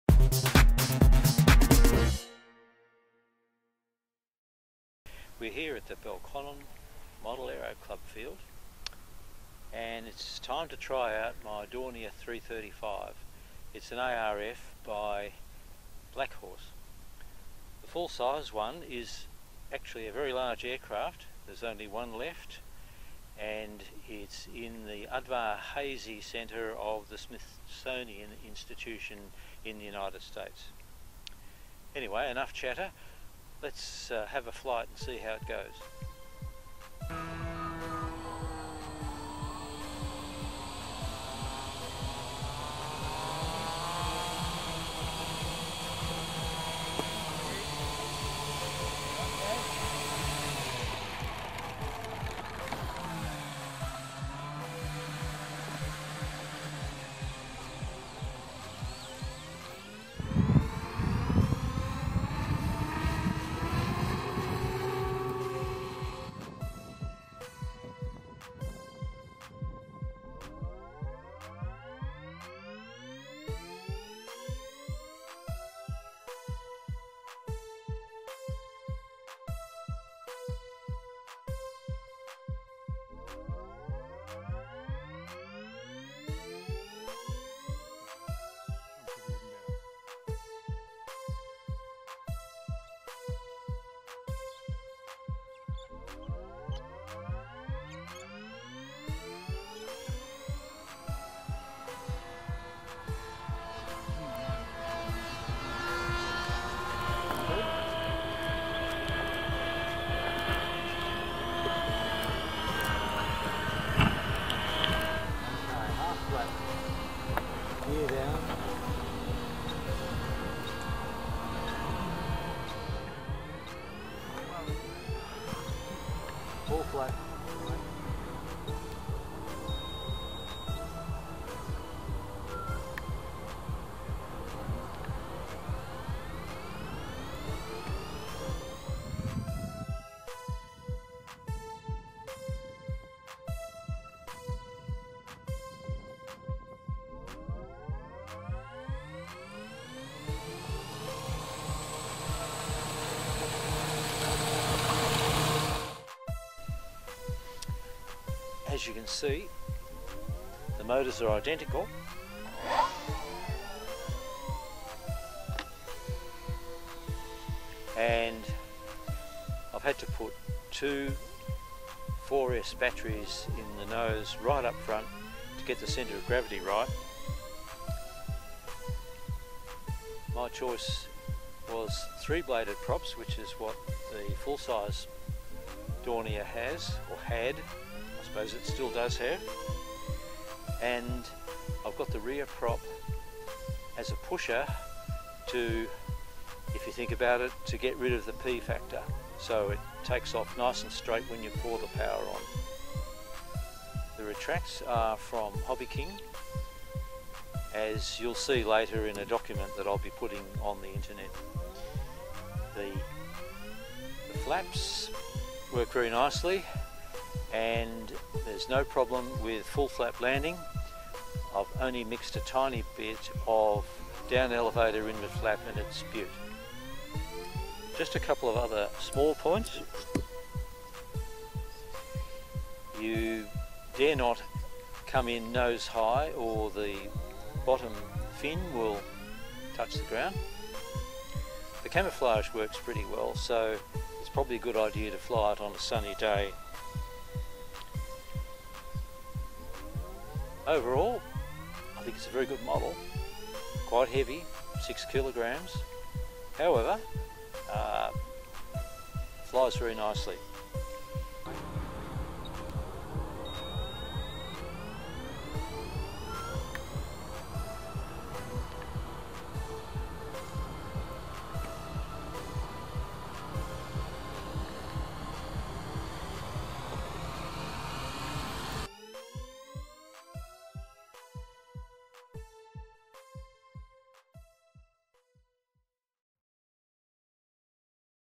Dornier 335
Flying the Dornier 335, an ARF by Black Horse.  Also, advice for prospective purchasers of the model.